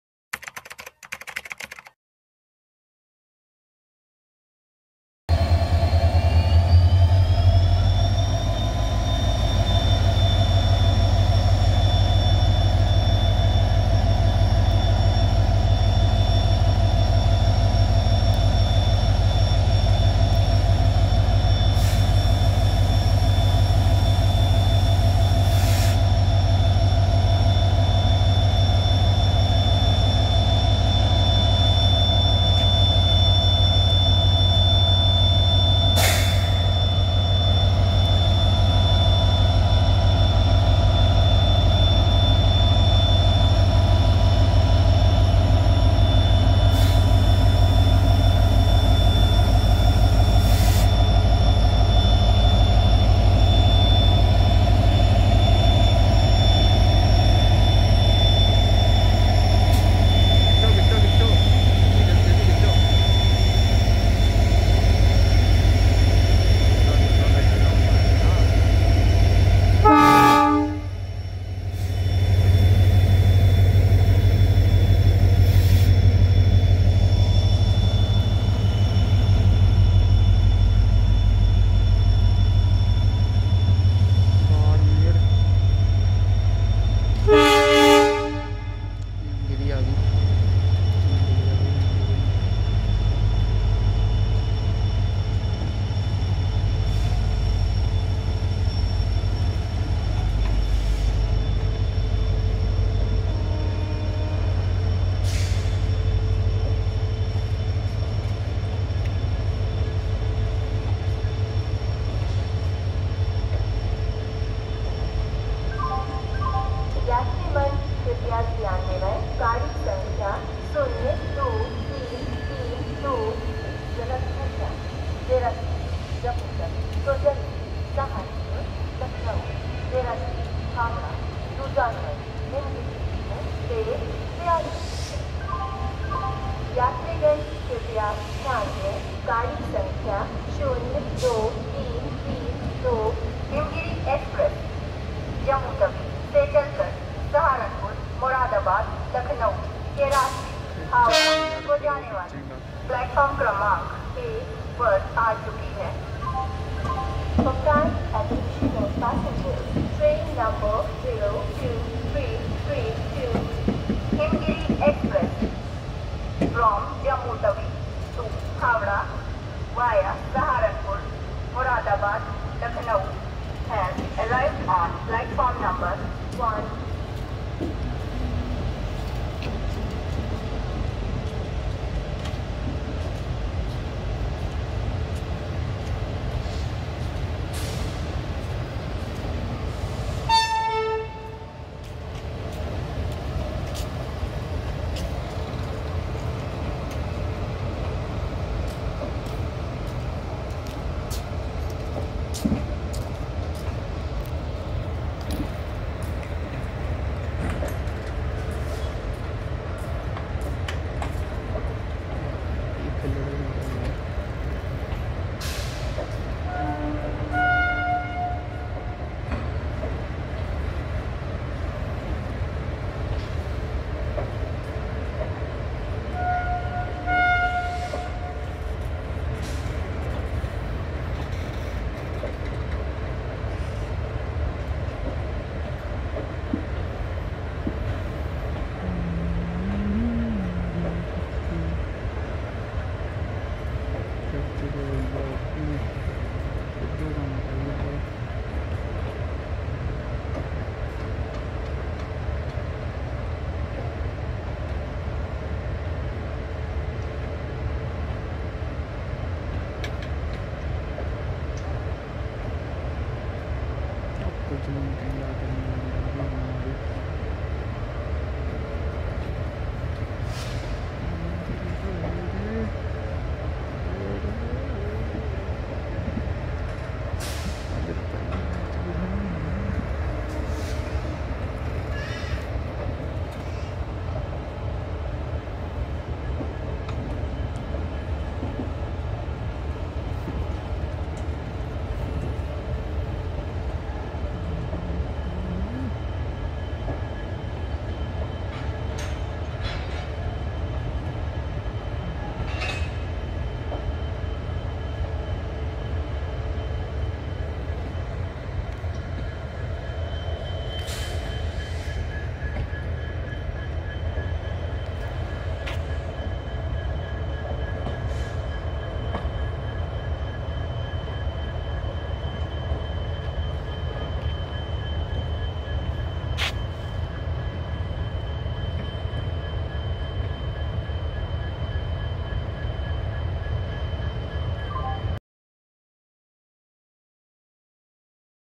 First Run Of (04646) JAT-JSM Shalimar Express After 10 Months Departing From Ludhiana jn
First Run Of (04646) JAT-JSM Shalimar Express After 10 Months Departing From Ludhiana jn With LDH WDP 4D.

NOTE- This video was captured Today morning at 3:20 am i.e 5-6 hours before publishing.

Details of the train :-

Frequency : Four Days A Week

Total Distance : 1560 Kilometres

Route : Via - Ludhiana,Ambala cantt, Saharanpur, Muzaffarnagar, Merrut city, Ghaziabad, Delhi jn, Gurgaon, Rewari, Alwar, Bandikui,  Jaipur & Jodhpur.

Rake type : ICF

Loco : Ludhiana WDP 4D